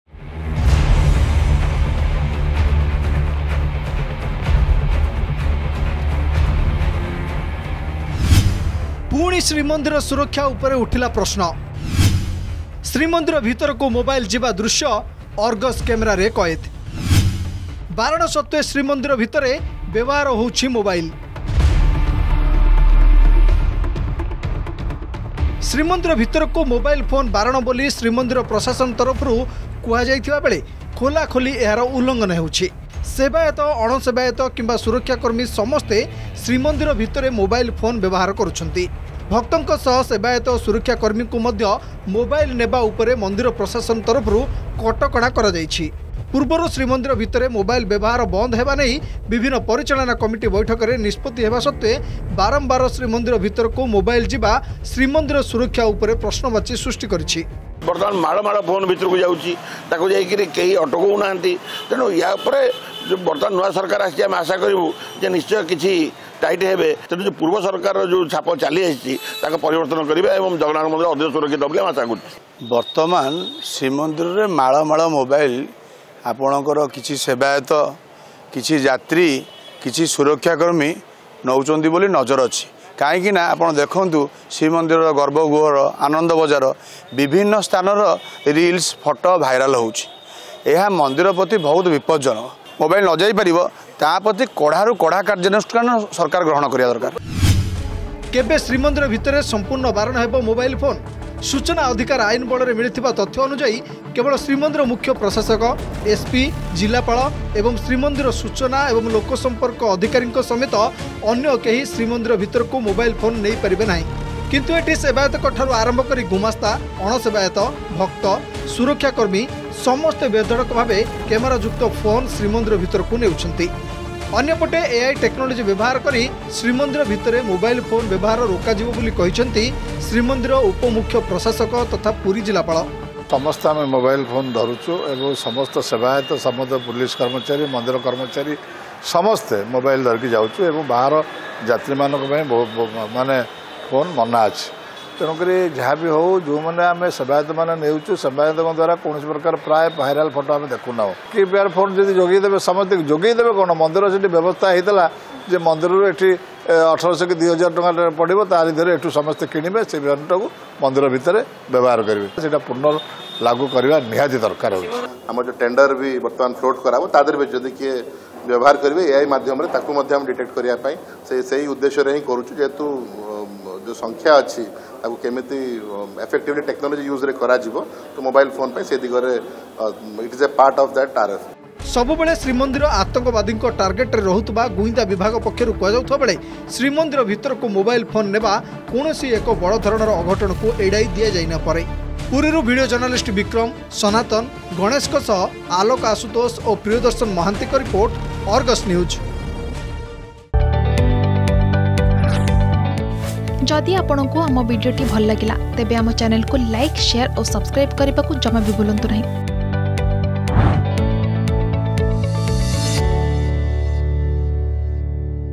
ପୁଣି ଶ୍ରୀମନ୍ଦିର ସୁରକ୍ଷା ଉପରେ ଉଠିଲା ପ୍ରଶ୍ନ | Srimandira Protection
ପୁଣି ଶ୍ରୀମନ୍ଦିର ସୁରକ୍ଷା ଉପରେ ଉଠିଲା ପ୍ରଶ୍ନ । ଶ୍ରୀମନ୍ଦିର ଭିତରକୁ ମୋବାଇଲ ଯିବା ଦୃଶ୍ୟ ଅର୍ଗସ କ୍ୟାମେରାରେ କଏଦ । ବାରଣ ସତ୍ତ୍ବେ ଶ୍ରୀମନ୍ଦିର ଭିତରେ ବ୍ୟବହାର ହେଉଛି ମୋବାଇଲ ।